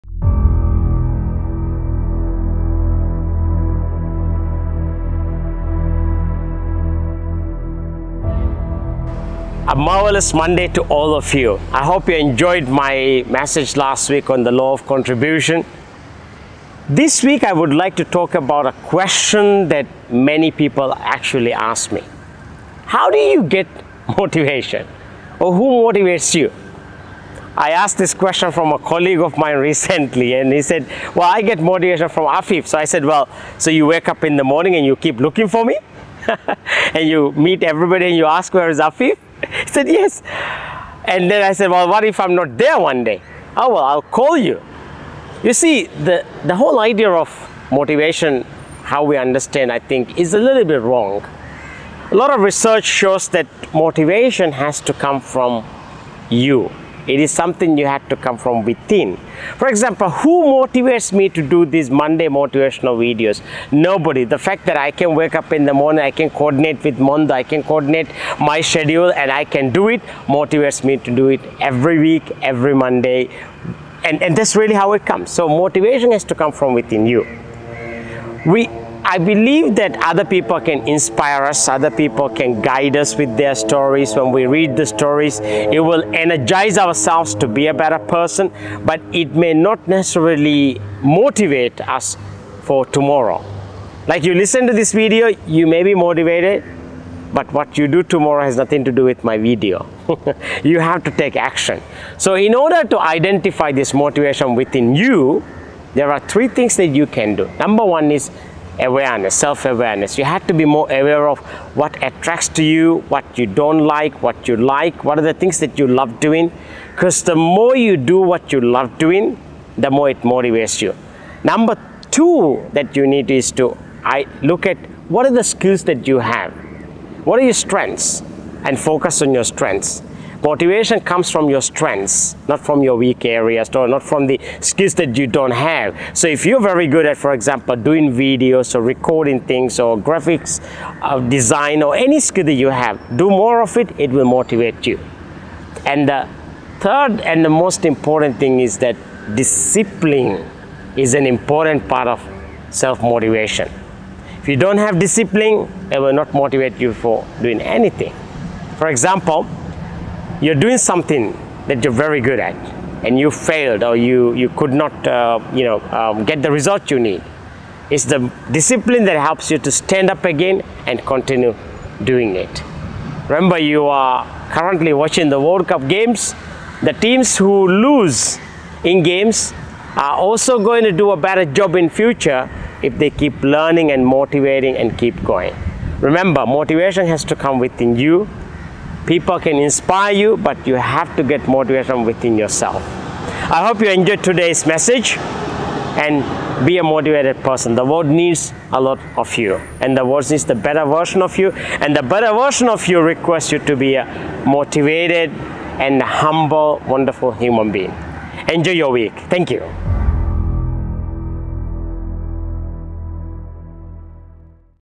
Monday Motivation with Afeef (23): Who Motivates Us?
Motivation is something that has to derive within us. Others can encourage, inspire and share their energy and enthusiasm, which may motivate us to believe, and take specific actions. But the real motivation to get things done and move forward has to come from us.

It is said motivation is like taking your daily bath. You have to reflect and drive your motivation daily. The fact that you can do what you do best must drive your motivation.
Nothing ever great was achieved in human history just because someone motivated someone. It all happened because someone found the motivation, a higher calling, and purpose within themselves, believed in it, took the right action and did what they needed to do. The more steps you take towards your goal will motivate you.